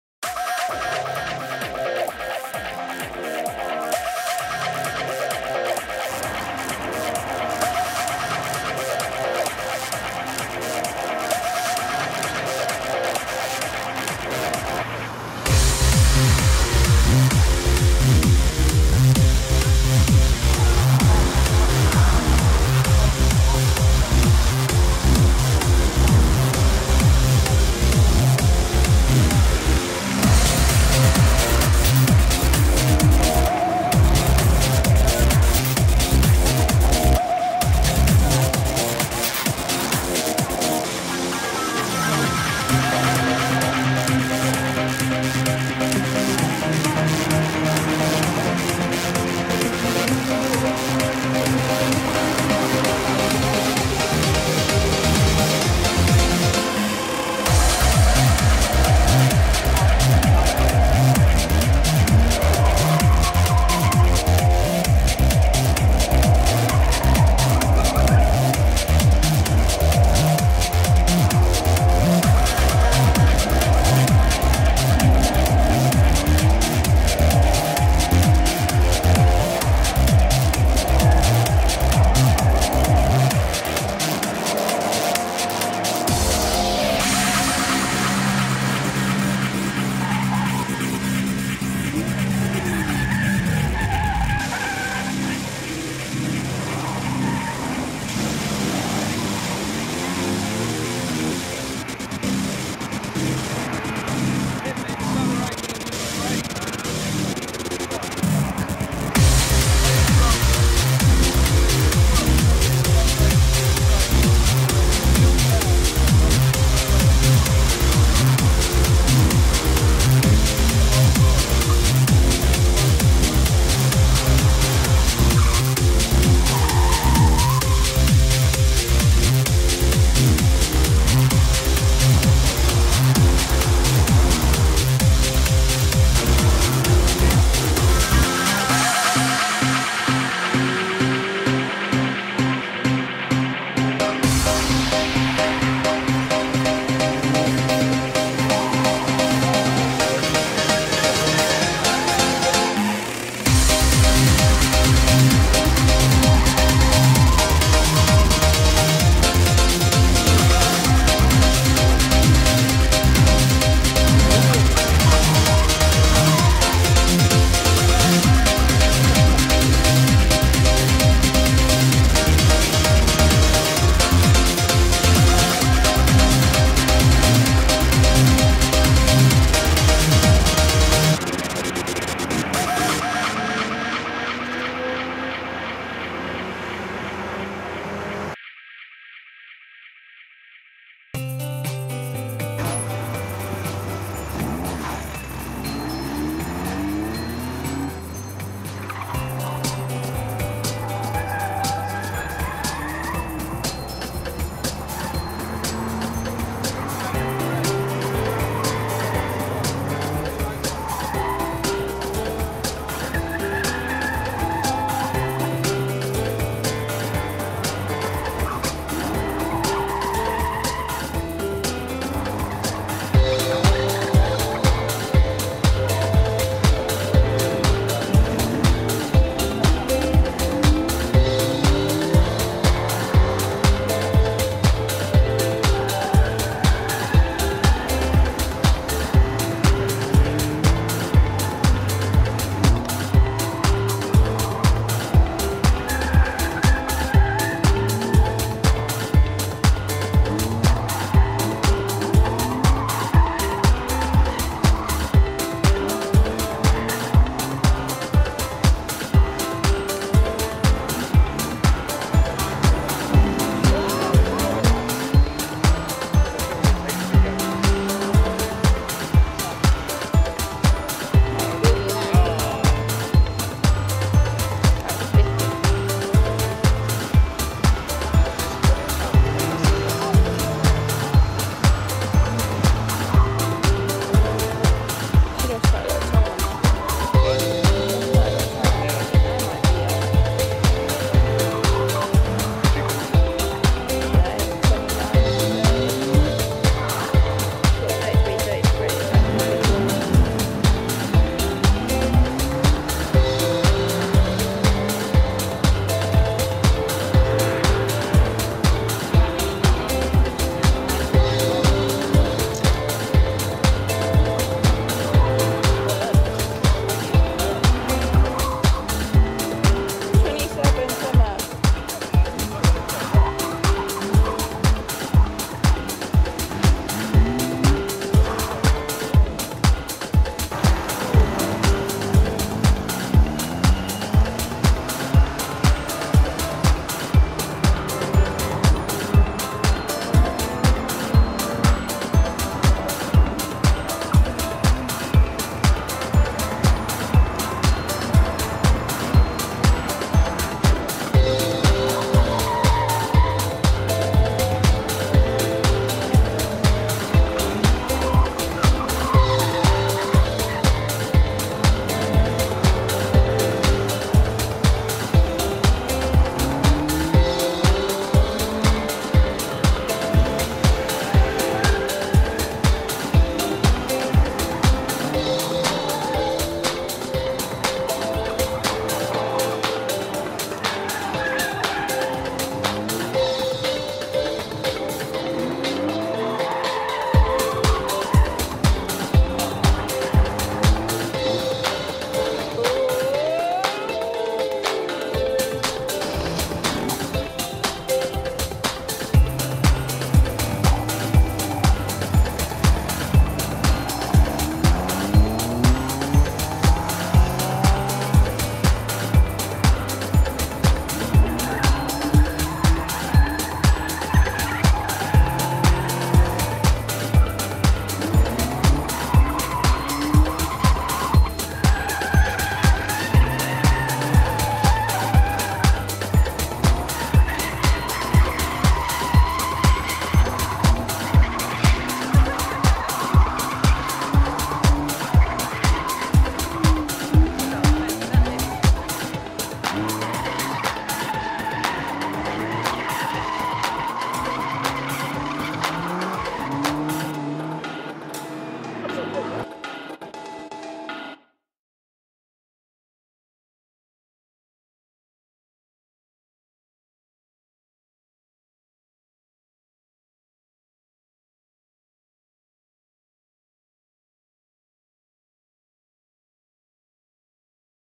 Caterham Slalom Day - Brands Hatch - 8/5/2013
A more exciting version of day I had at Brands Hatch on a Caterham Experience Slalom Drift day were you do timed runs around various cone set-ups. The 1st half (approx. up to 3mins) is kinda highlight bits, spins, etc... then the 2nd half is showing full runs.

I've done about 6 other track / supercar experiences and for fun and entertainment, you can't beat this!!!